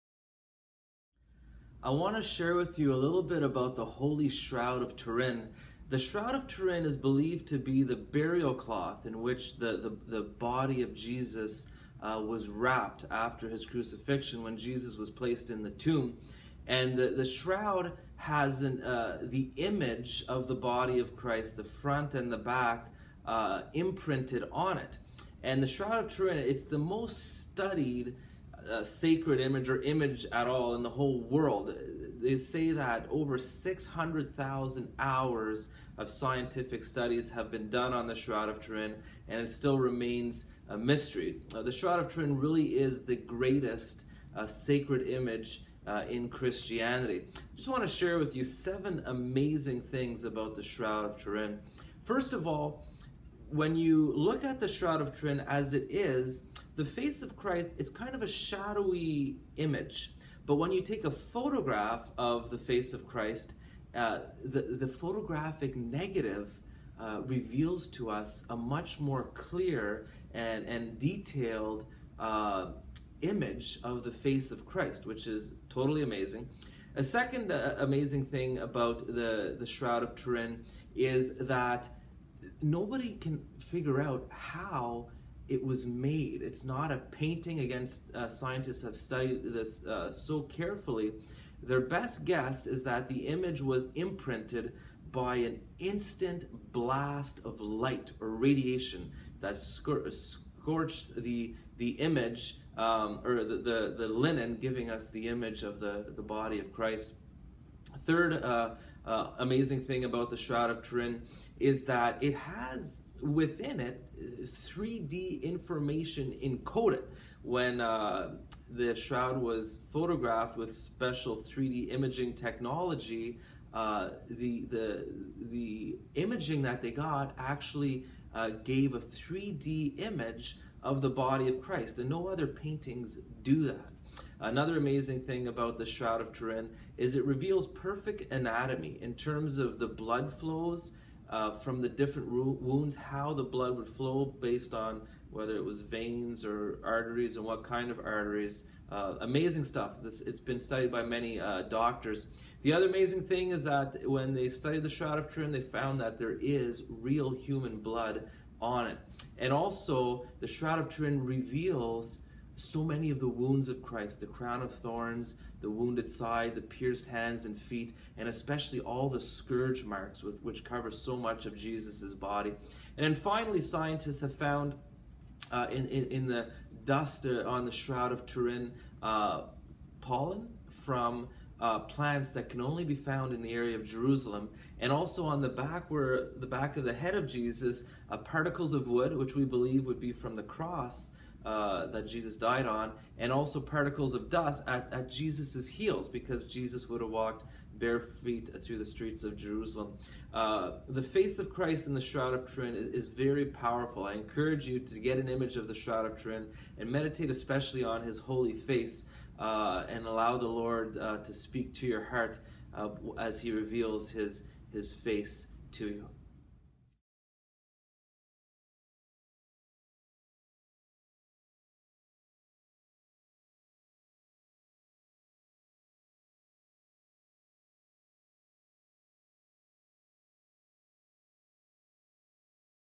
Fr. Mark Goring - 7 amazing things about the Shroud of Turin
They will look on him whom they have pierced, they will mourn for him as one mourns for an only child, and they will grieve for him as one grieves over a firstborn.
Zechariah 13:10